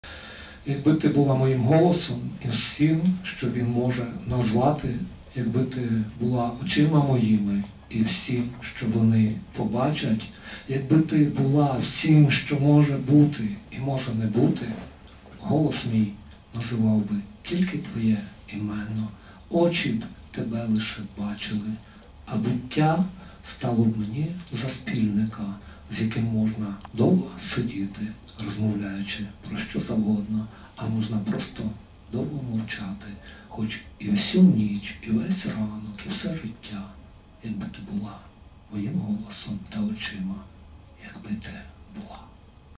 Костянтин Москалець Якби ти була моїм голосом
Якби ти була моїм голосом,
І всім, що він може назвати.
Якби ти була очима моїми,
І всім, що вони побачать.

Якби ти була усім, що може бути,
І може і не бути.
Голос мій називав би тільки твоє імено,
Очі, тепер лише очі. (ост 2 рядки – 2)

Якби ти була...
Якби ти була...

Творчий вечір поета Костя Москальця. Презентація збірки «Поезія Келії».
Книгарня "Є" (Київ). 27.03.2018